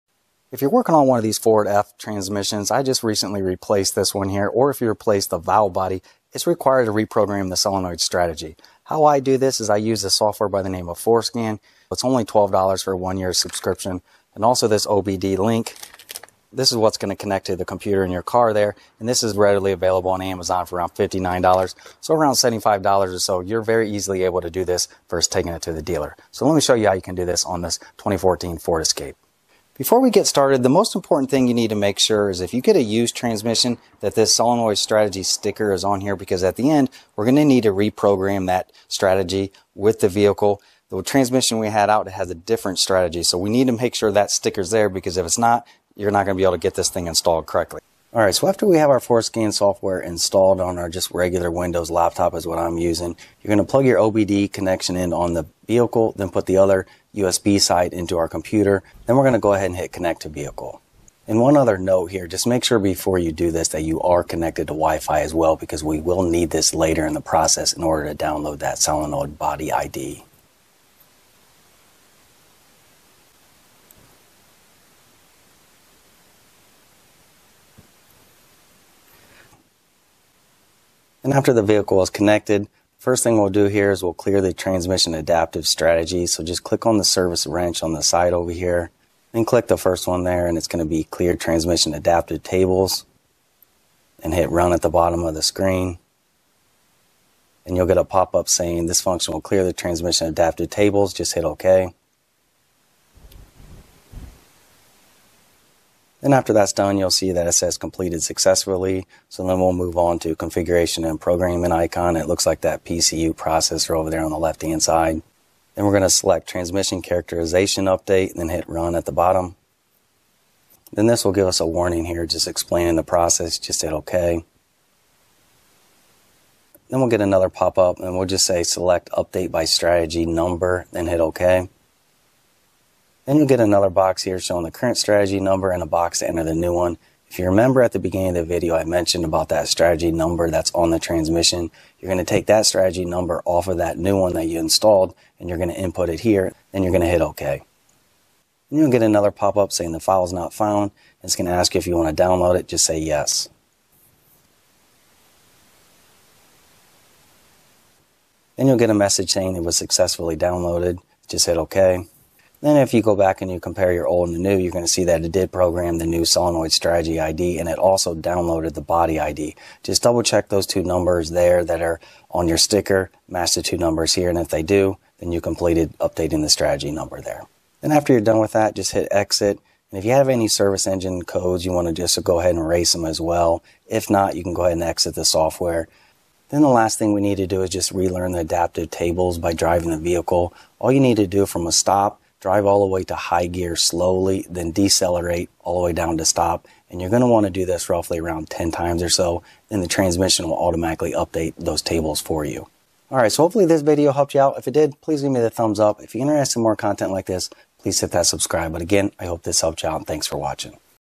How to Program Ford Escape 6F35 Transmission Solenoid Strategy [Required if Replaced Transmission!]
In this video we show how to clear the transmission adaptive tables and program the transmission solenoid strategy on a 2014 Ford Escape by using FORScan with an OBDLink EX.  Any time you replace the valve body or transmission on a Ford F transmission, it is required to clear the transmission adaptive table and reprogram the solenoid strategy.  Although it may sound hard, it's a straightforward process that definitely doesn't require a trip to the dealer as you can do it yourself at home!  This is the same process for various other Ford transmissions on vehicles from 2011+ including Ford Explorer, Edge, Fusion, F150, and more!

Items discussed in this video to review specs and/or purchase.  Thank you for the support!

OBDLink EX FORScan OBD Adapter:

Chapters:
00:00 - Intro
00:31 - Solenoid Strategy Sticker Location
00:49 - Connect to Vehicle
01:22 - Clear Transmission Adaptive Tables
01:49 - Transmission Characterization Update
03:07 - Relearn Transmission Adaptive Tables
03:24 - Wrap Up